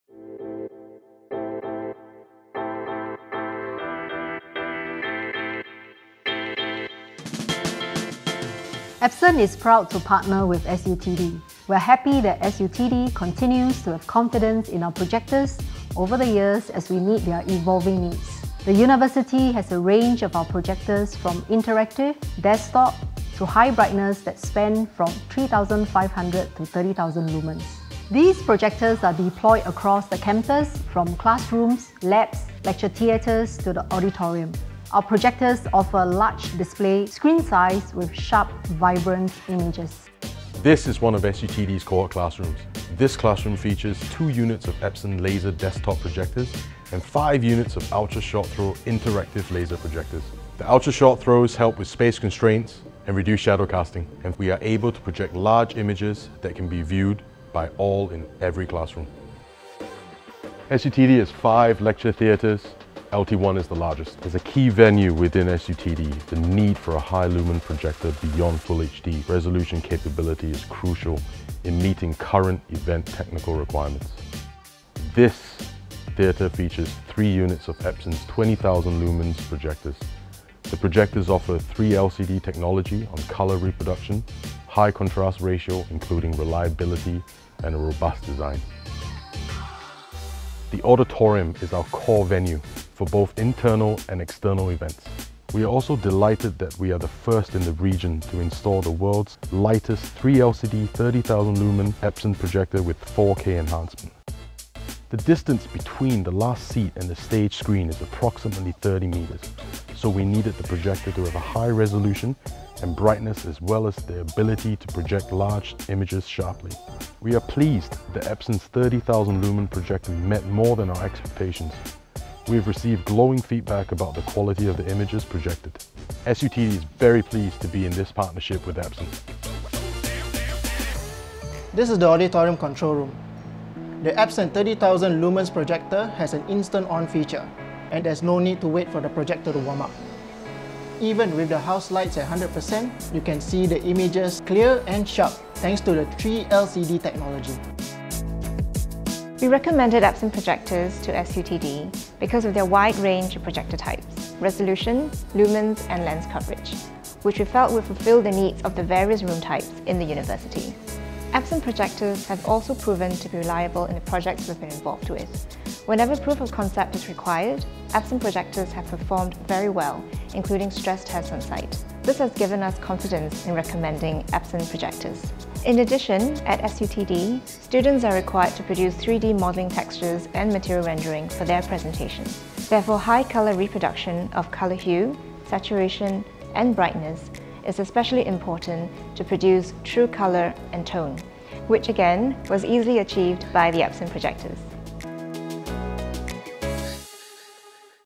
Máy chiếu Epson - lựa chọn của trường Đại học Công nghệ và Thiết kế Singapore (SUTD)
Singapore University of Technology and Design (SUTD) là trường đại học công lập đứng vị trí thứ tư tại Singapore. Trường đã lựa chọn các sản phẩm máy chiếu Epson để phục vụ cho công tác giảng dạy nhờ vào sự đa dạng về các dòng máy, cũng như độ phân giải, độ sáng và lựa chọn ống lens đáp ứng được các nhu cầu khác nhau của từng loại phòng học khác nhau tại trường.

Đừng bỏ lỡ những video mới nhất từ Epson Việt Nam.